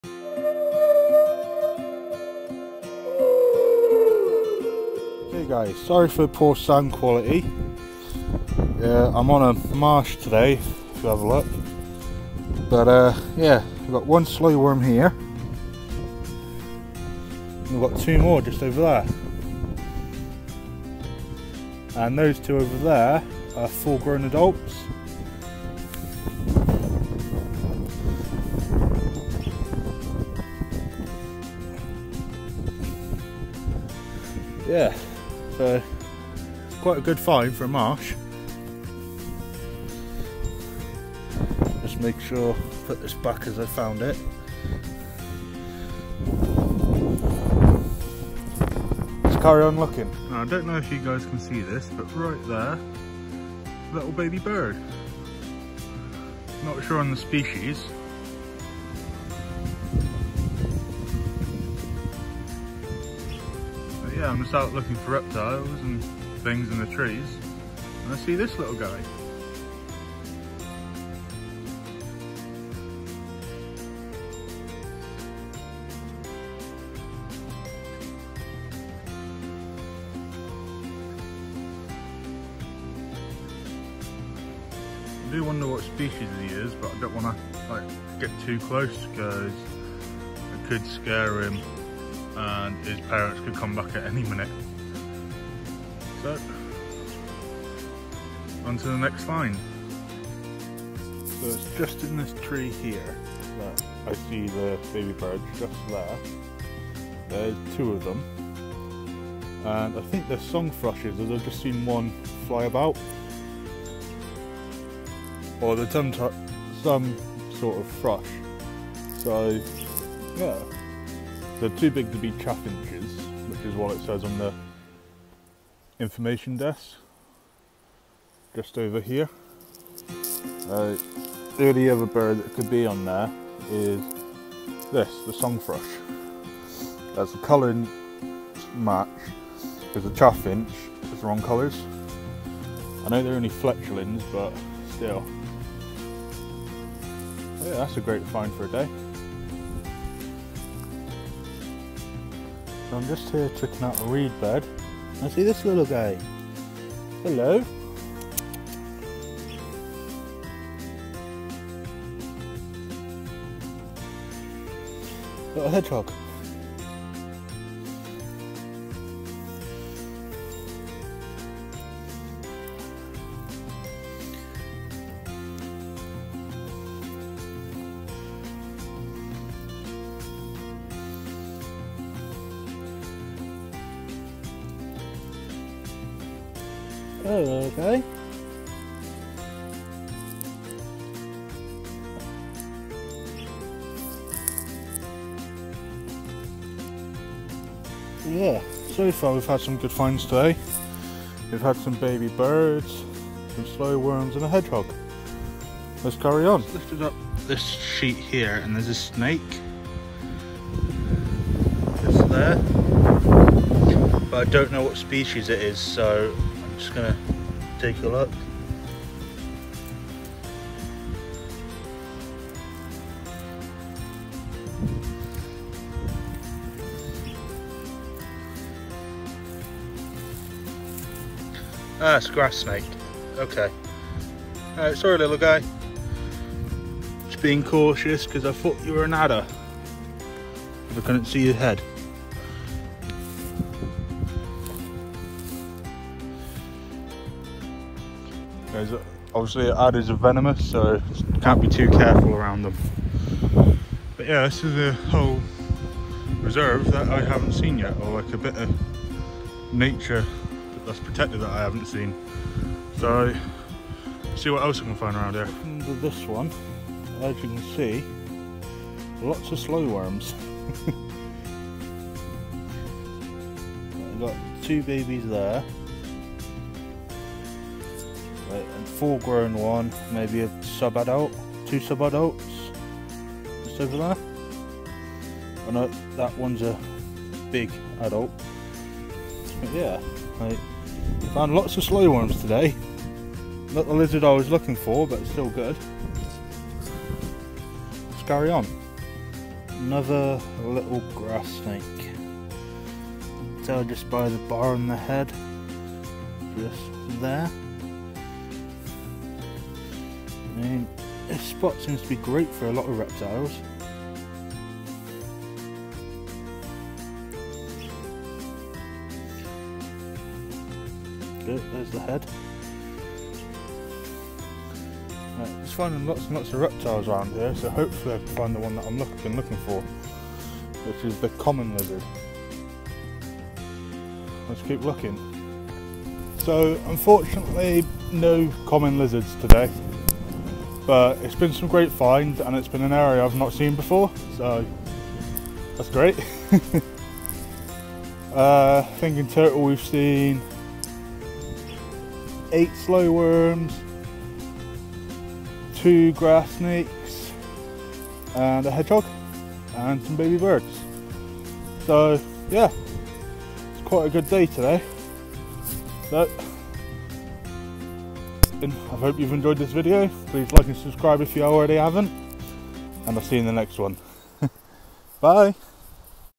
Birds & Reptiles
So today I’ve been out at a new locations and found loads of animals.